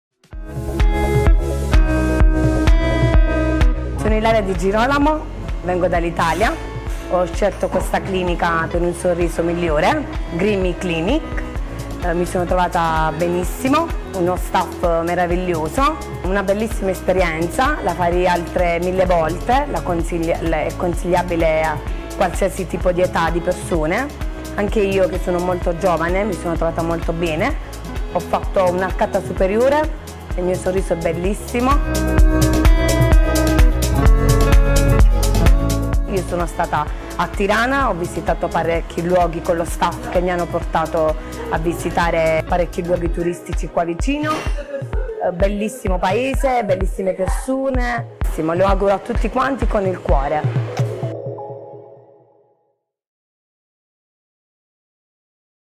NOUVEAU SOURIRE - Gremi Clinic
✅NOUVEAU SOURIRE
Ilaria Digirolamo a maintenant les yeux pleins de joie, car un sourire pétillant illumine son visage. Des émotions qu'il ne pensait pas pouvoir réessayer. Regardez le résultat final